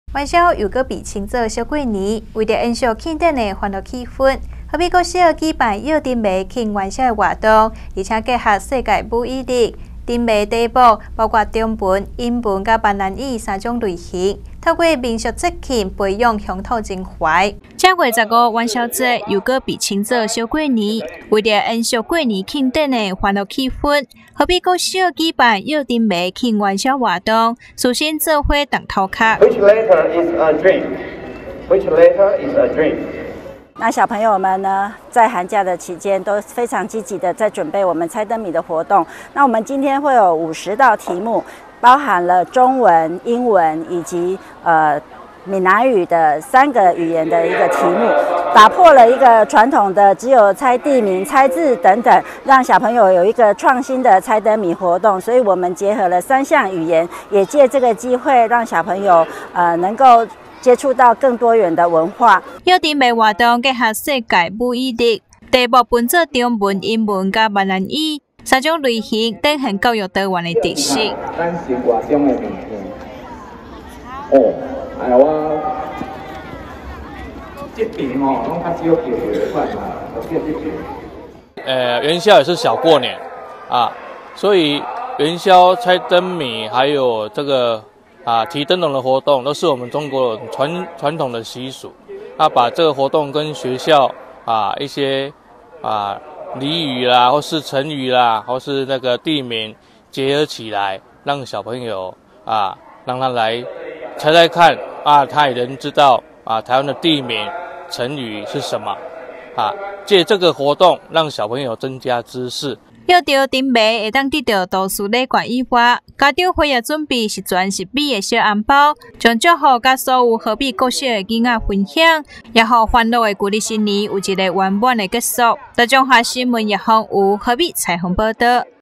1130222 和美國小猜燈謎慶元宵 喜領十全十美小紅包
元宵又被稱做小過年，為延續慶典的歡樂氣氛，和美國小舉辦猜燈謎慶元宵活動，並結合世界母語日，燈謎題目包括中文、英文及閩南語三種類型，透過民俗節慶，培養鄉土情懷。